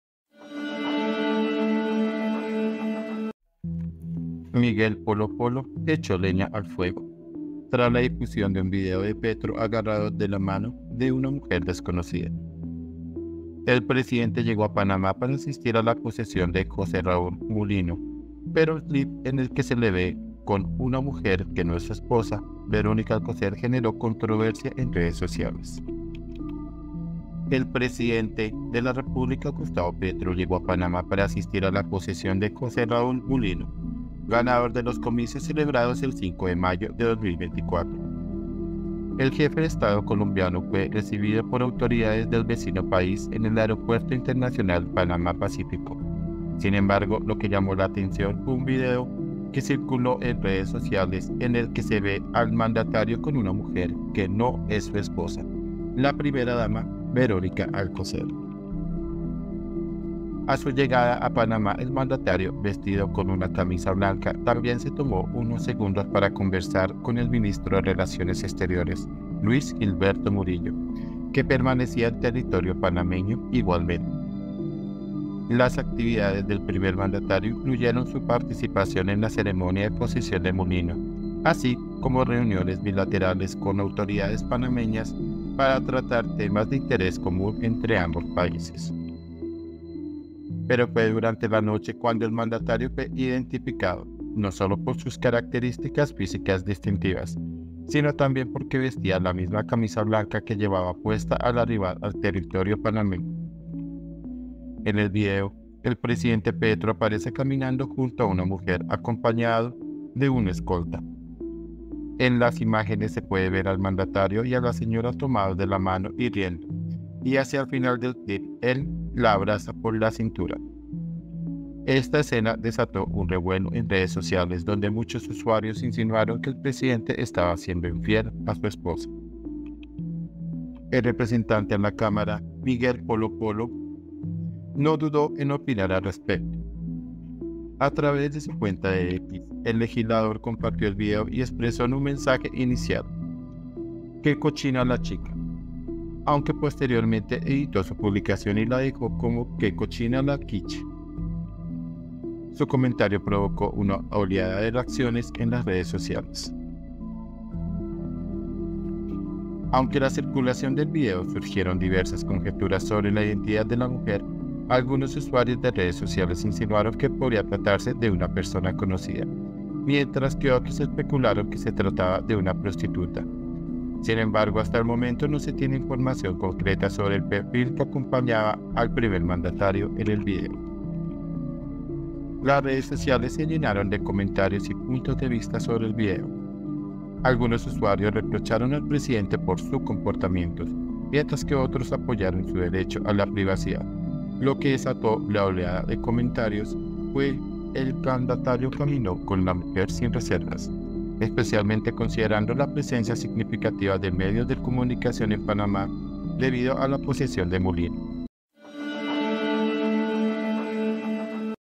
¿INFIDELIDAD PRESIDENCIAL? ❌ Miguel Polo Polo ‘echó leña al fuego’
Miguel Polo Polo ‘echó leña al fuego’ tras la difusión de un video de Petro agarrado de la mano de una mujer desconocida
El presidente llegó a Panamá para asistir a la posesión de José Raúl Mulino, pero un clip en el que se le ve con una mujer que no es su esposa, Verónica Alcocer, generó controversia en redes sociales
INFOBAE